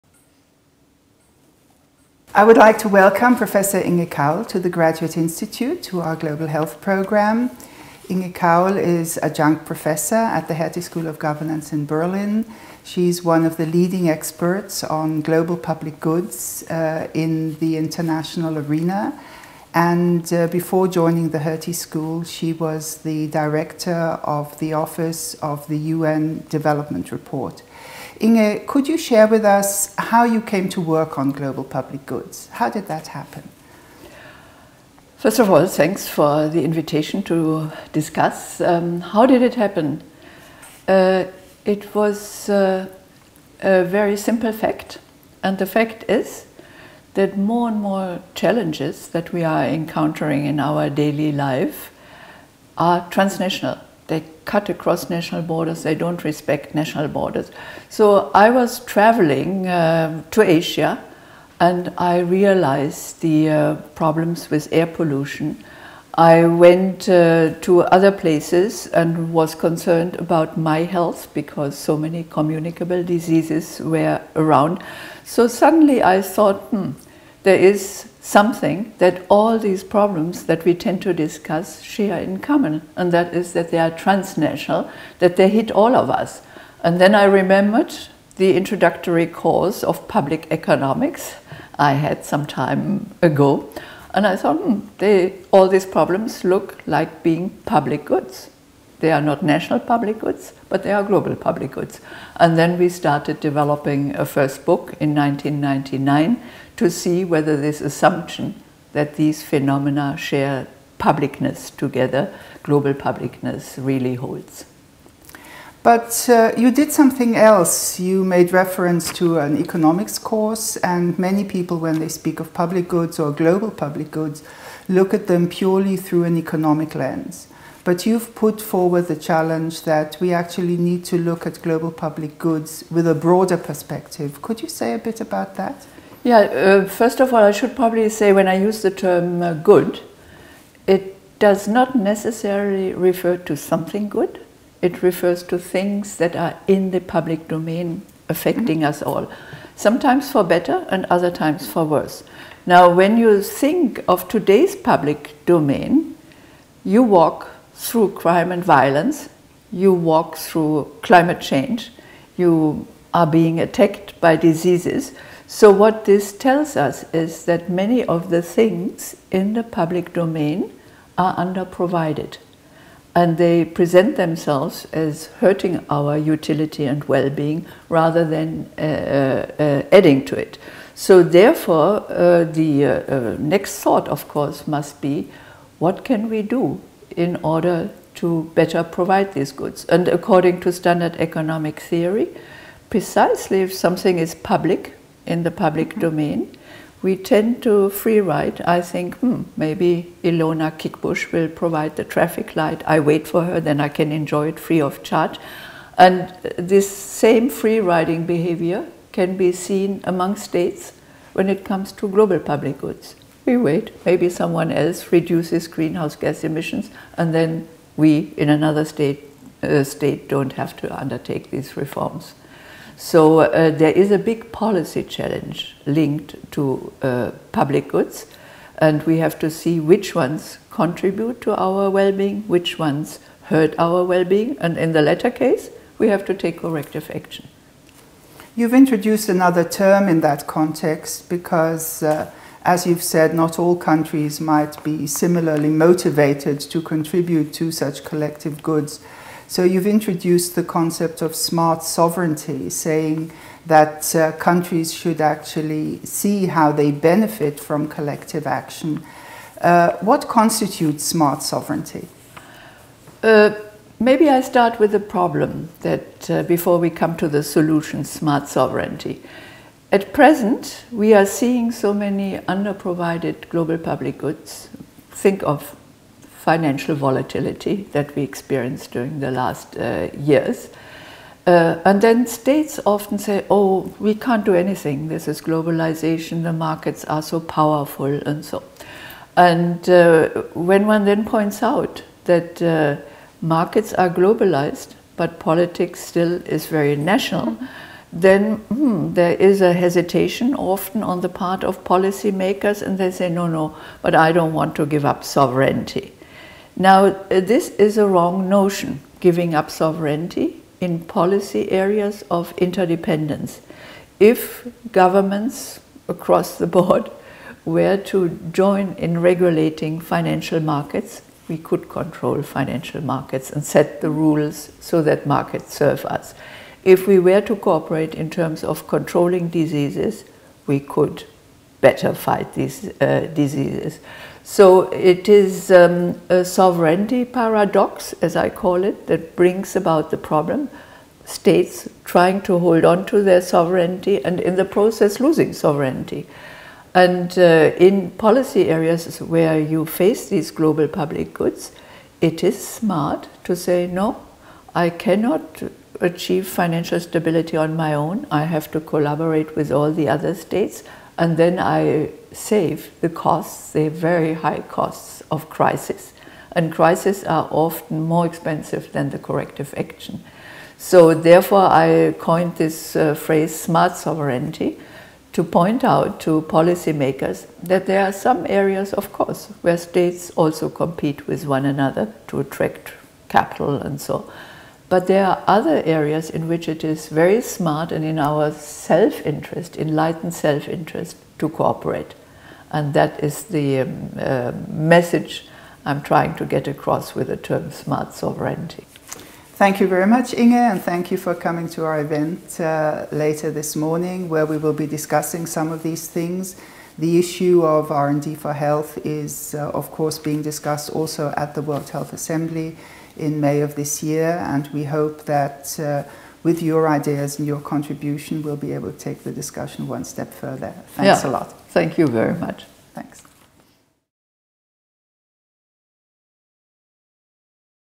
Inge Kaul
Inge Kaul, Adjunct Professor, Hertie School of Governance, interviewed by Ilona Kickbusch, Director of the Global Health Programme, The Graduate Institute.